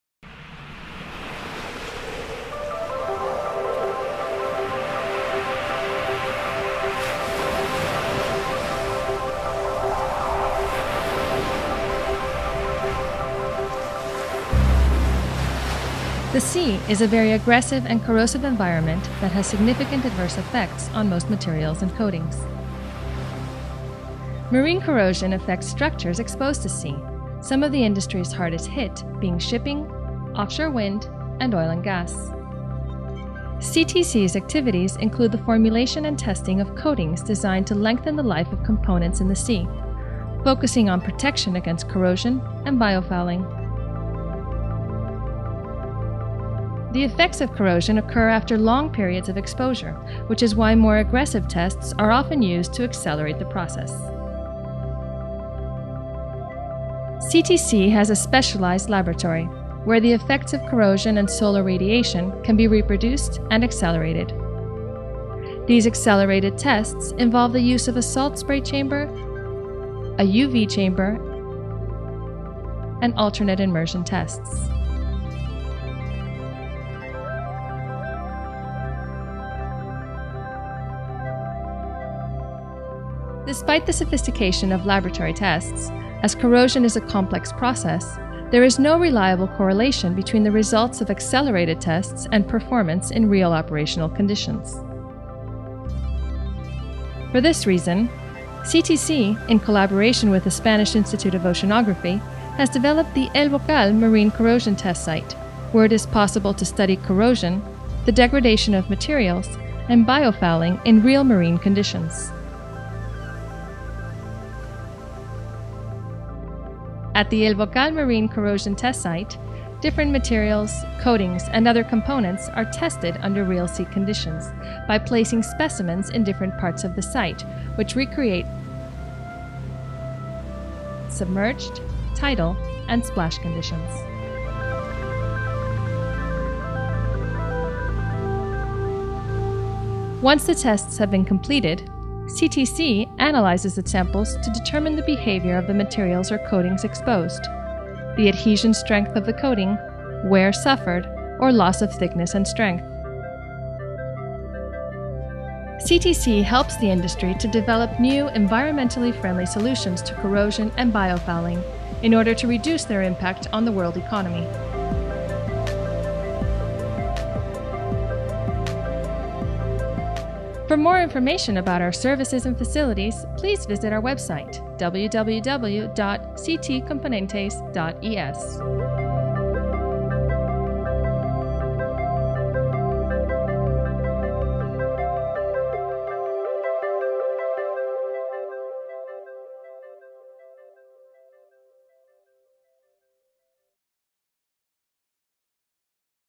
Marine Laboratory – MCTS El Bocal
English version of the video explaining Marine Laboratory "MCTS El Bocal". A facility, developed by  CTC, with the aim of making marine corrosion tests under real conditions. The MCTS El Bocal is the only laboratory of its kind that exists in Spain.